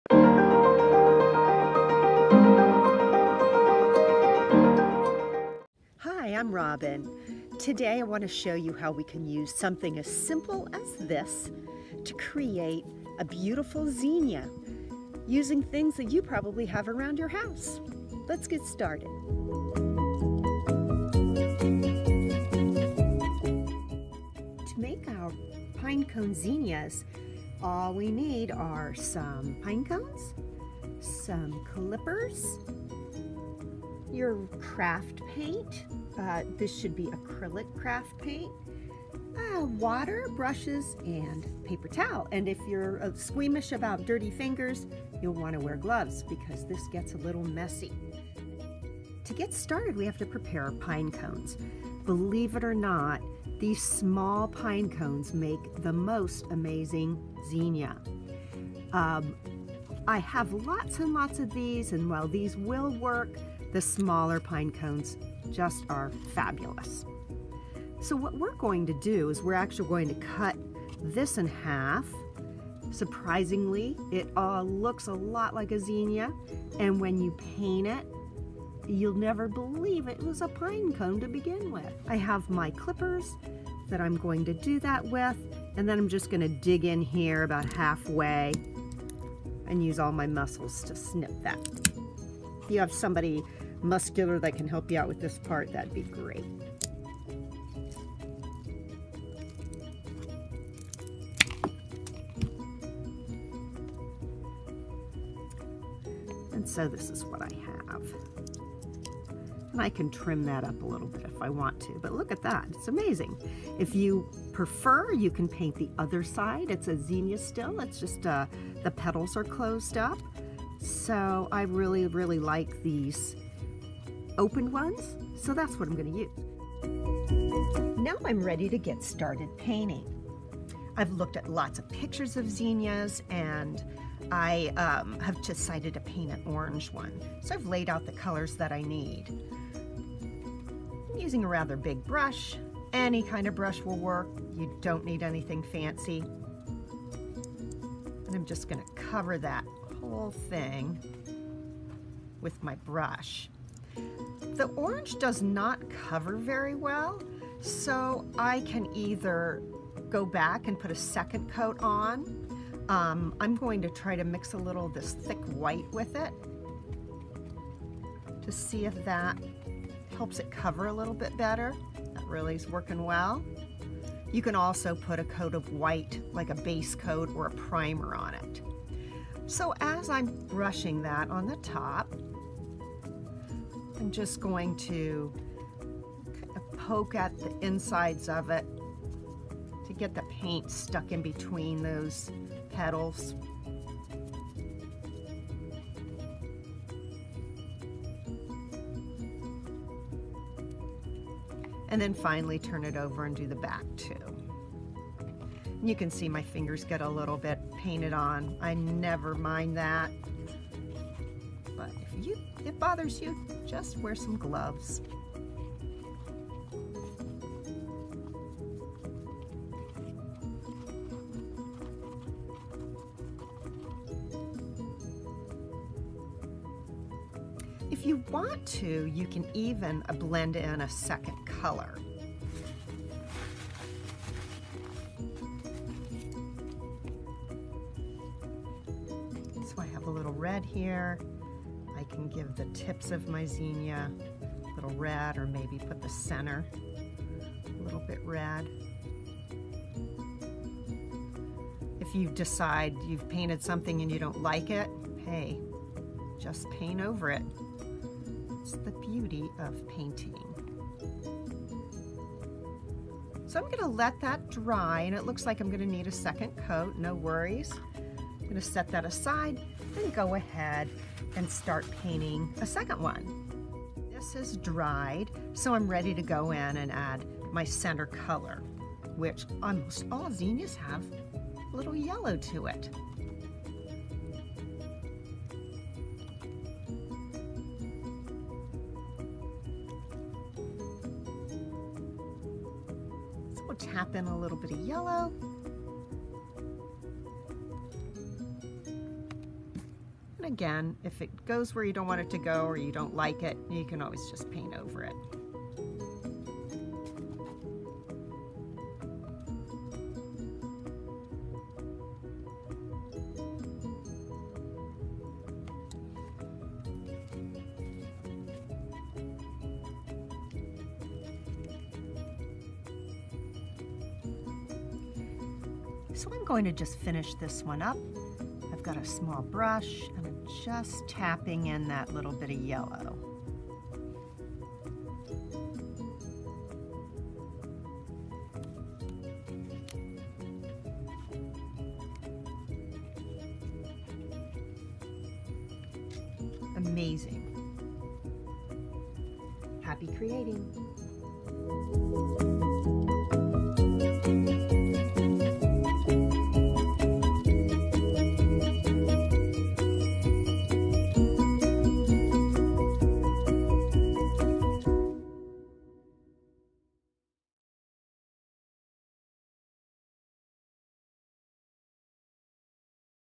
How to Make Pine Cone Zinnias with Robin Clay
Using what you may have at home, Robin Clay, art instructor at SourcePoint, guides you through this fun and colorful nature project using pine cones to create zinnias that can be used in a number of decorations.